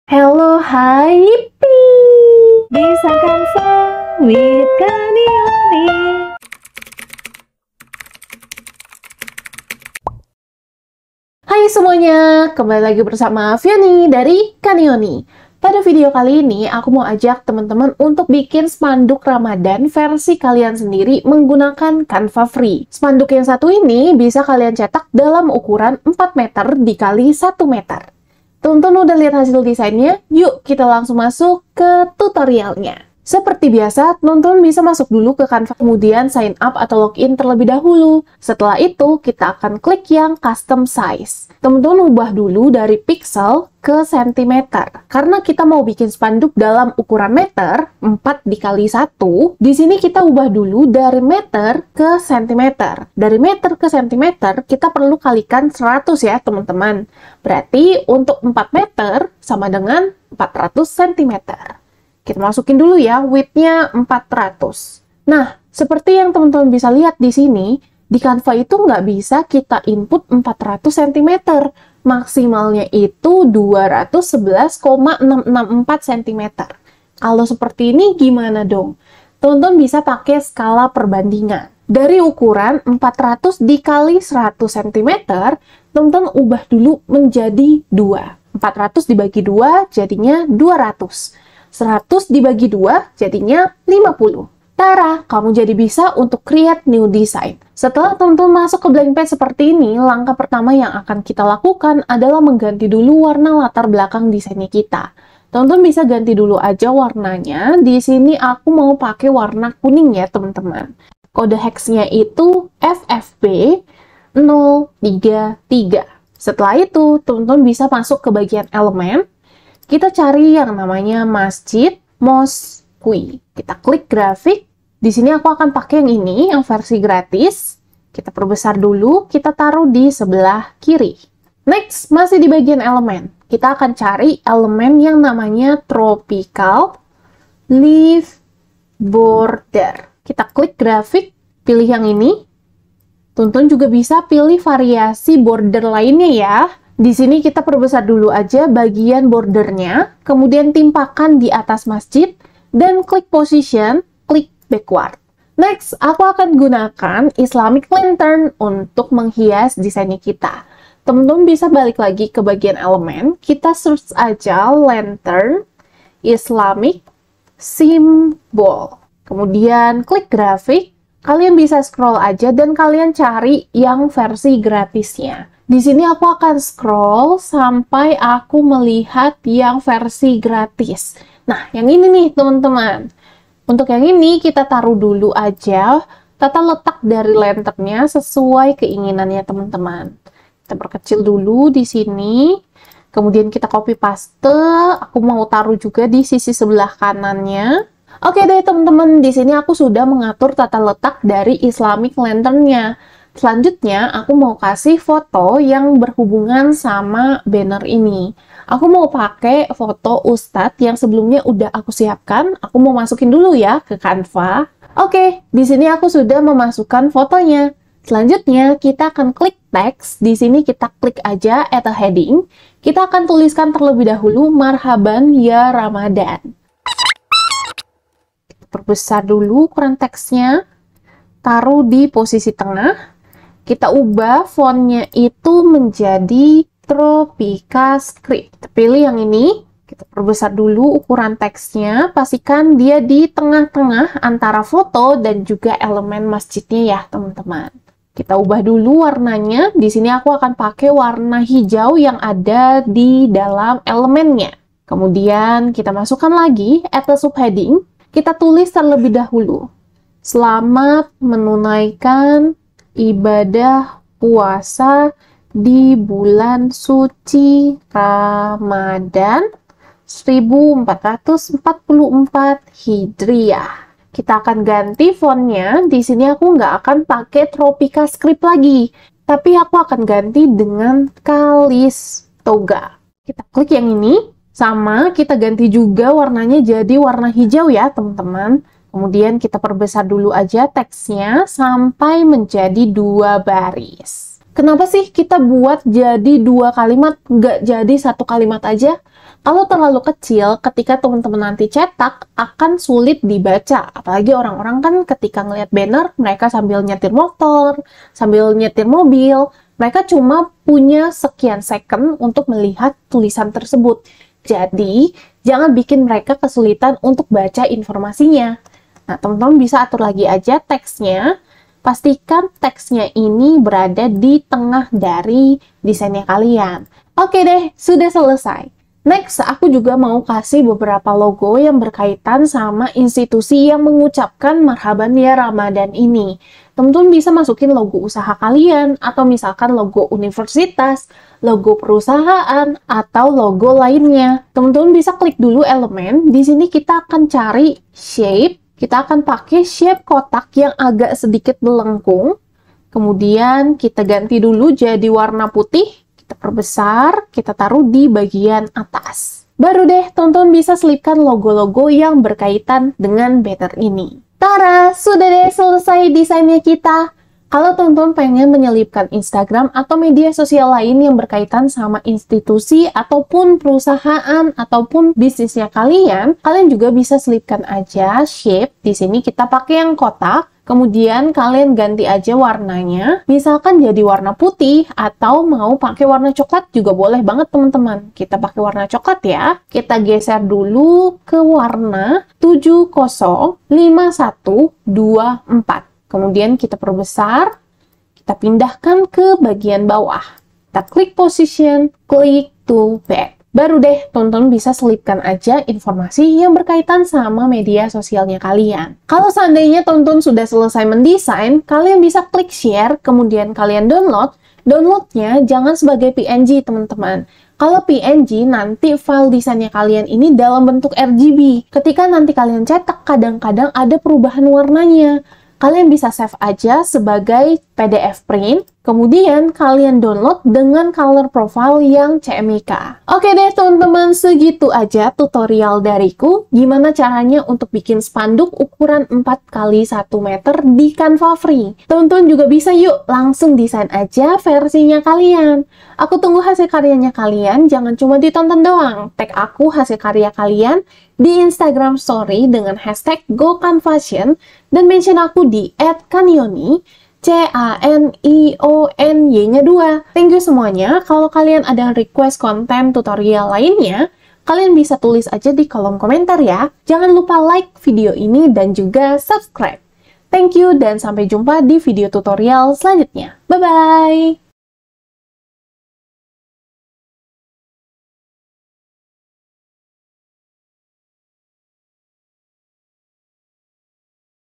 Tutorial Desain Spanduk Lebaran 4 Meter x 1 Meter di Canva FREE
Hai-halo dan annyeong!
Selamat datang di Youtube Canionyy yeaayy~

Mau buat spanduk desain dengan ukuran besar yang bisa diikat ketika acara ataupun di tepi jalan raya? Memangnya menggunakan Canva bisa? Tentu saja bisa! Yuk kita buat spanduk ucapan selamat menunaikan ibadah puasa berukuran 4x1 meter di Canva FREE dan kalian hanya perlu menggunakan Canva FREE aja jadi yuk praktekin tutorialnya, dan posting dengan hastag gocanvation di media sosial kamu. Jangan lupa tag aku di IG atau tiktok @canionyy ya.

Mau request tutorial? Boleh banget dong! share aja request kamu di kolom komentarnya ya ^^

Mau langganan Canva PRO? atau mau dapet ebook gratis?

Semoga kalian suka sama tutorialnyaaa~